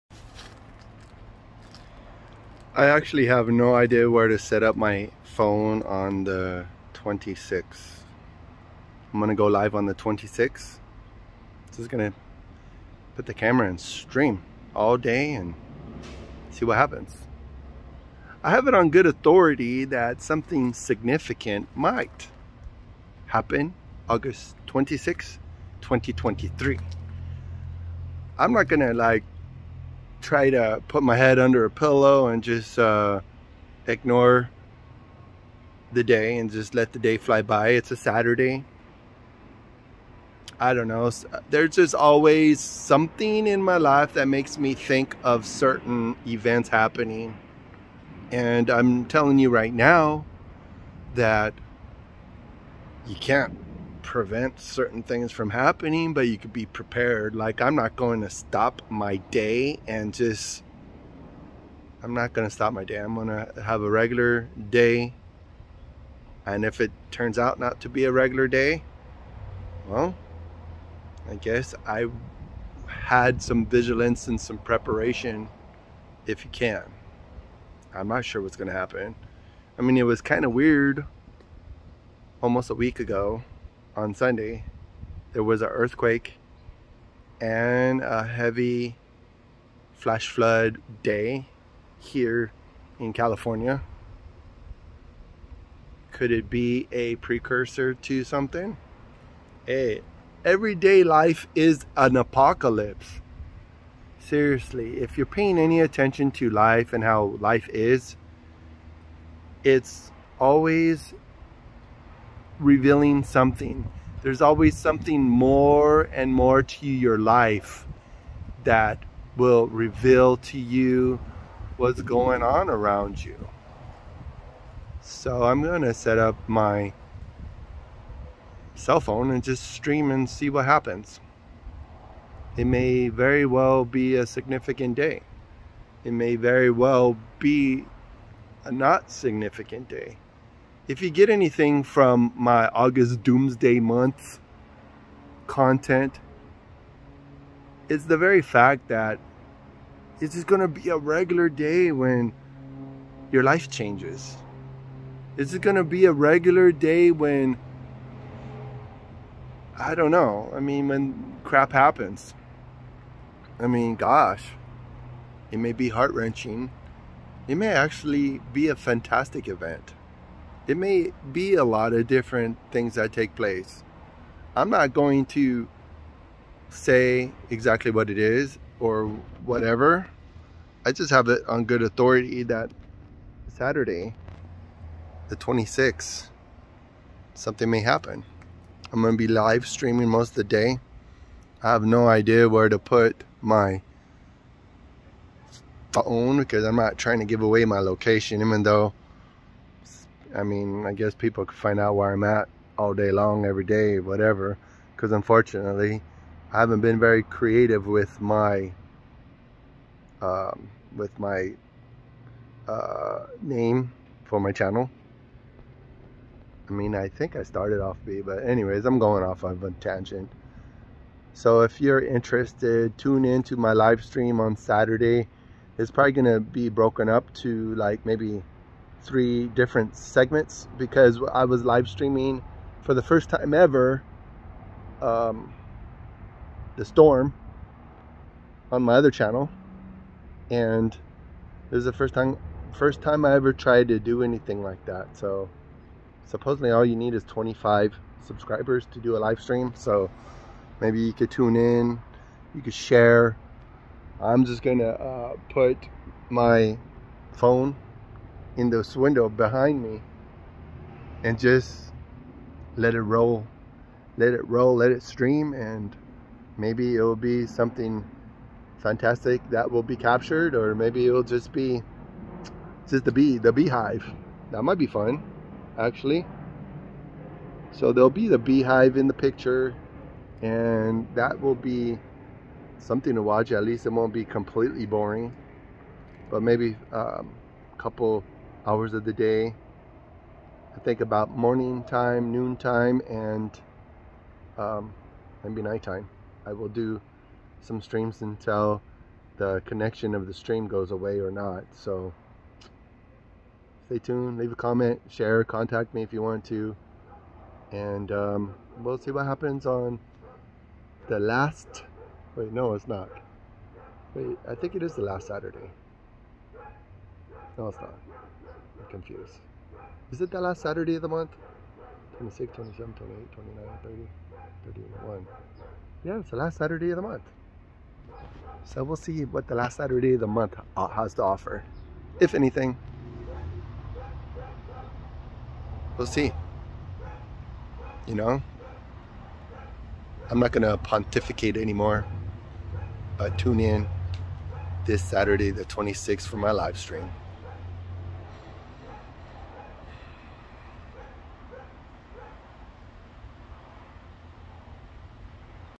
Last Saturday of August it's going down? 2023
August 26th I expect things will change.  I'm not a prophet or son of a prophet, but I just make observations.